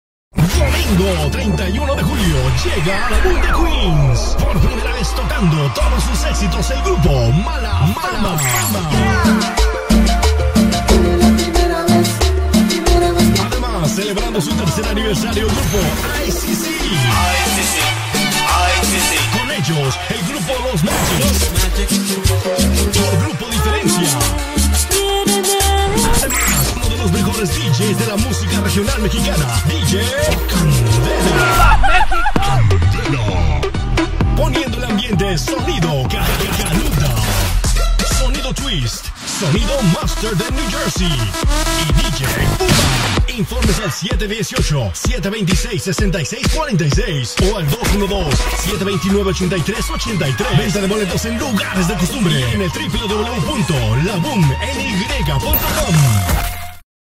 bailazo sonidero julio 31 club la boom
Domingo 31 de Julio Llega A LaBoom De Queens Ny Por Primera Vez Tocando Todo Sus Exito Grupo Mala Fama Ademas El Grupo ICC Celebrando Su 3 Aniversario, Con ellos el Grupo Los Magix y Grupo Diferencia Ademas uno de los mejores Djs de La Musica Regional Mexicana Dj Kandela poniendo el ambiente Sonido Caluda, Sonido Twist, Sonido Master de NJ y Dj Puma. Inf@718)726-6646 O @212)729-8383 ID REQUERIDO DAMAS + CABALLEROS 21+ NO lGORRAS NO PANUELOS NO JERSEY NO SHORTS